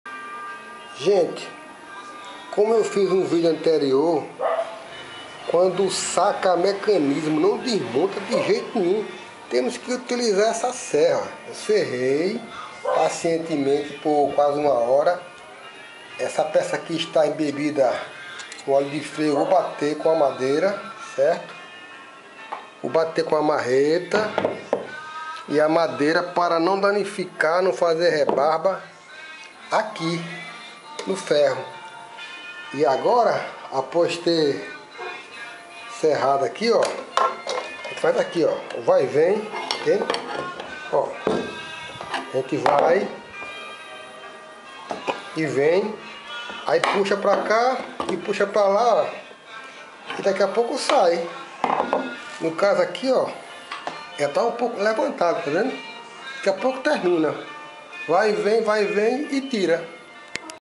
Não aconselho a ninguém SERRAR o tubo da lavadora brastemp,mas há casos que FAÇO...
NÃO ACONSELHO. É MUITO TRABALHOSO,MAS RESOLVE!